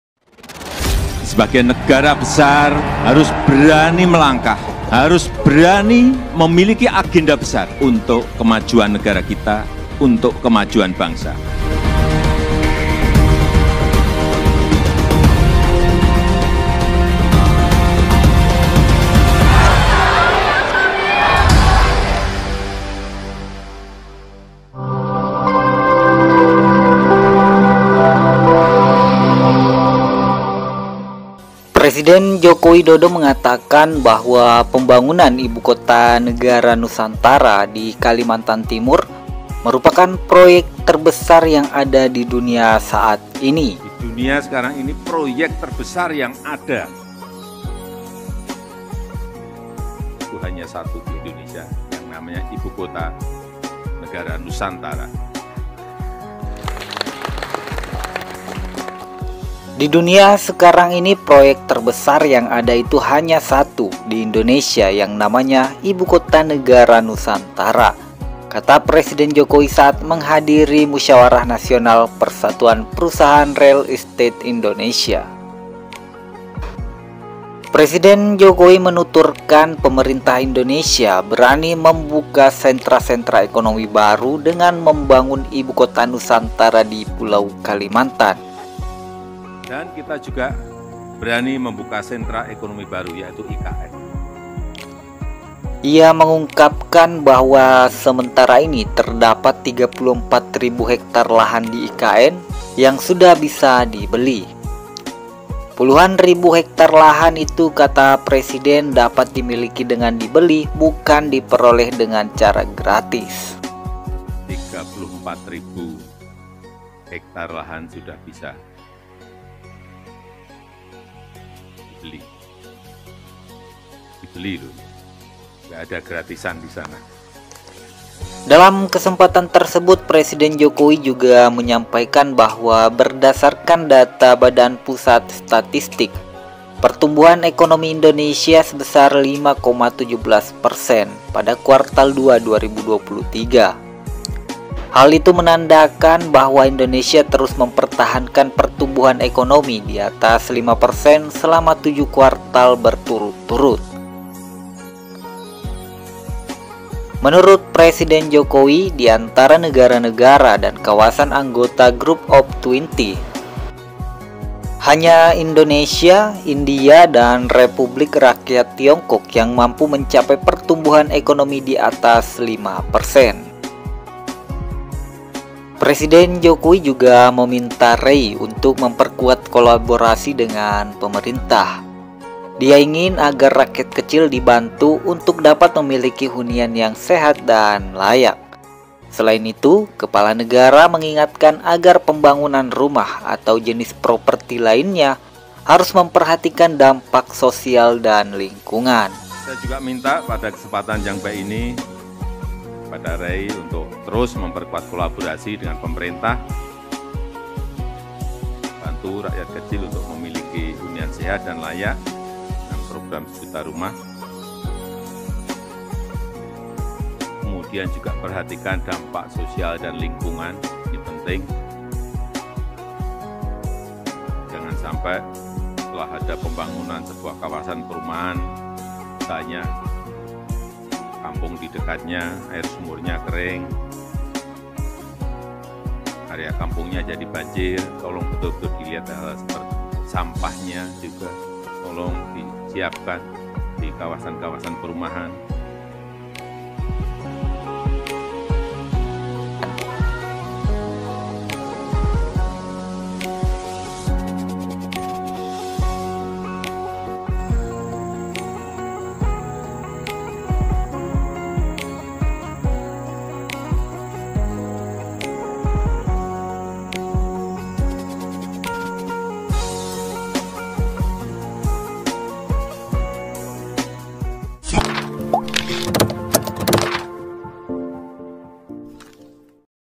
IKN HARI INI‼️ Pembangunan IKN Nusantara Proyek Terbesar Di Dunia Saat Ini
Pembangunan IKN Nusantara Proyek Terbesar Di Dunia Saat Ini

Statement tersebut disampaikan langsung oleh presiden joko widodo.

Pembangunan Ikn Nusantara di Kalimantan Rimur terus berjalan. Kini progresnya sudah mencapai 27 Persen.

Pembangunan IKN ditarget agar bisa pindah di Tahun 2024.

BorneoTube menyajikan informasi dihimpun dari berbagai media  kemudian dibuat kesimpulan. Hal ini Menjadi referensi untuk kita saling berbagi.